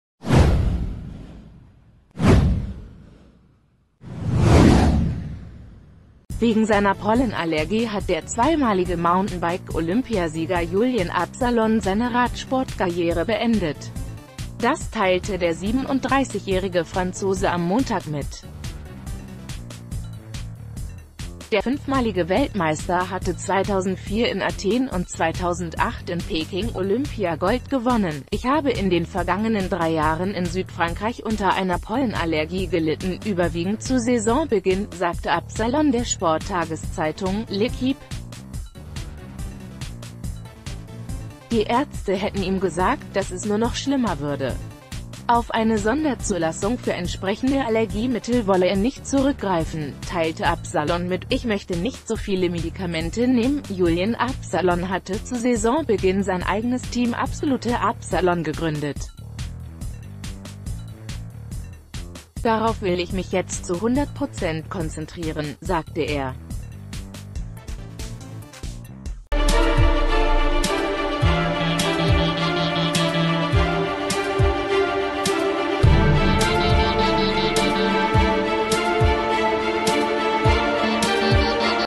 Wegen Pollenallergie: Olympiasieger beendet Karriere
Wegen seiner Pollenallergie hat der zweimalige Mountainbike-Olympiasieger Julien Absalon seine Radsportkarriere beendet. Das teilte der 37-jährige Franzose am Montag mit. Der fünfmalige Weltmeister hatte 2004 in Athen und 2008 in Peking Olympia-Gold gewonnen."Ich habe in den vergangenen drei Jahren in Südfrankreich unter einer Pollenallergie gelitten, überwiegend zu Saisonbeginn", sagte Absalon der Sporttageszeitung "L'Equipe". Die Ärzte hätten ihm gesagt, "dass es nur noch schlimmer würde." Auf...